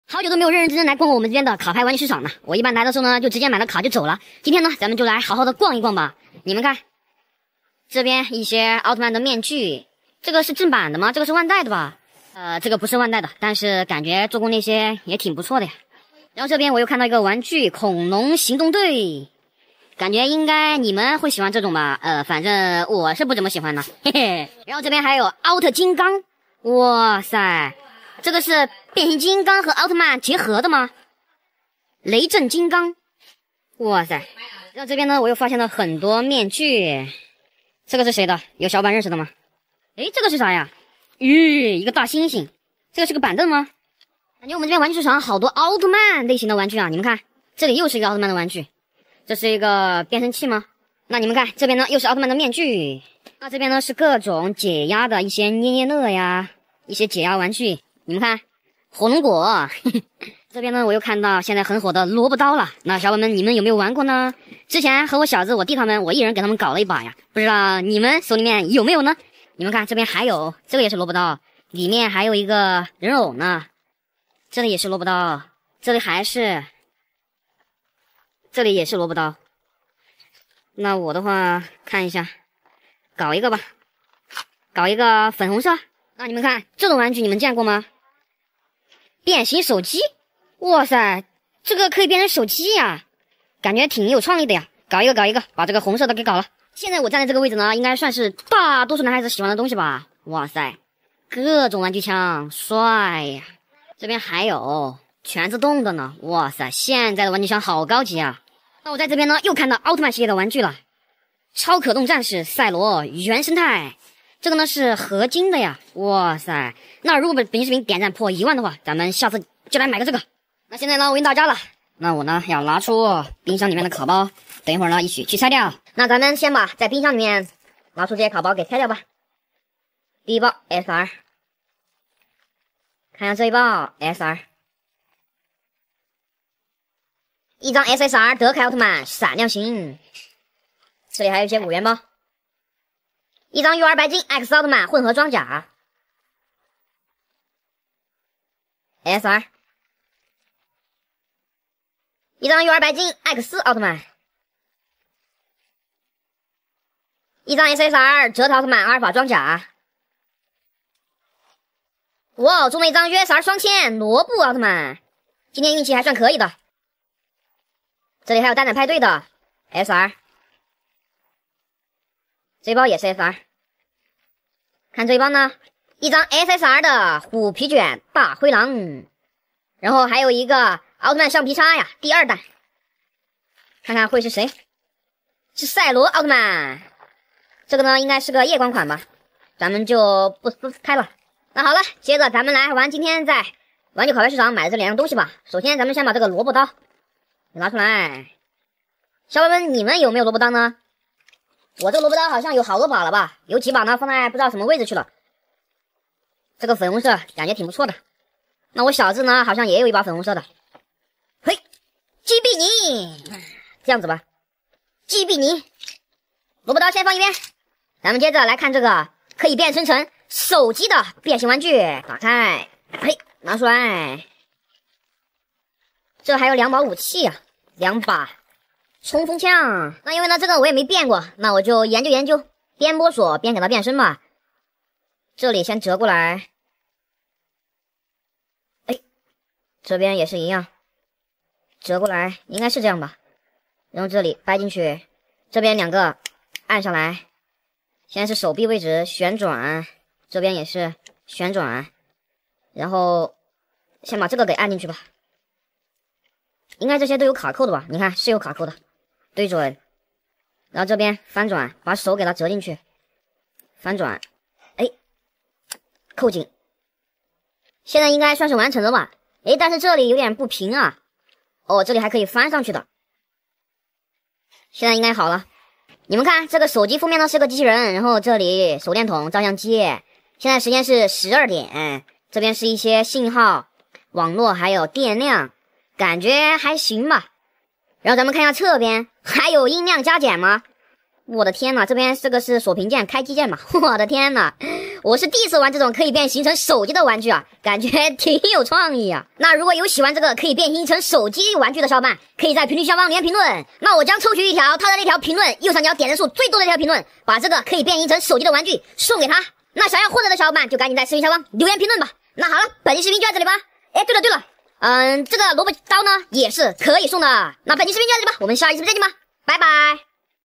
今天去逛玩具市场，买到了萝卜刀和可以变形成手机的玩具，真好玩【蒙蔽优斯】
请放心，本视频是一部原创拍摄的短剧，没有任何不良行为。 今天去逛玩具市场，买到了萝卜刀和可以变形成手机的玩具，真好玩
哈喽大家好 😁😁